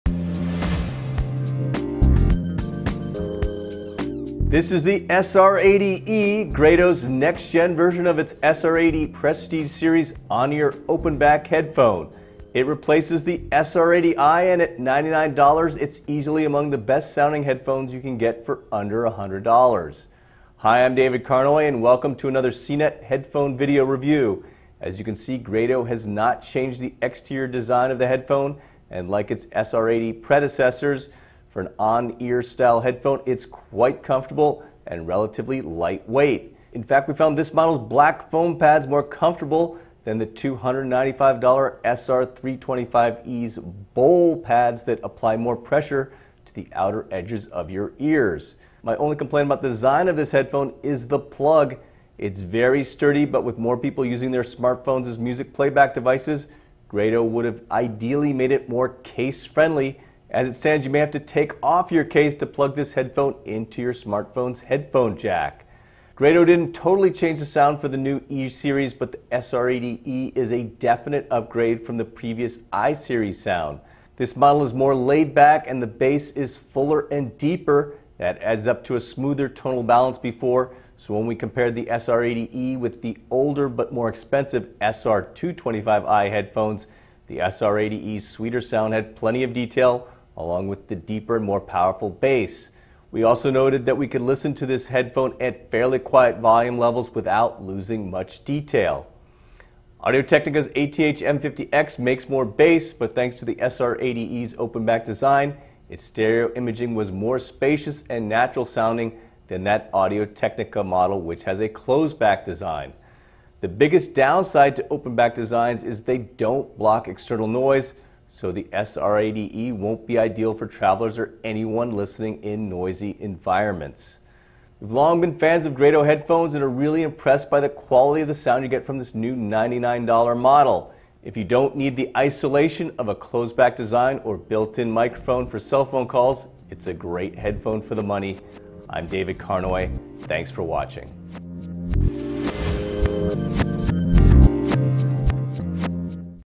Grado SR80e: A great-sounding headphone for under $100
Grado's comfortable open-back, on-ear model delivers high-quality sound for an affordable price of $99.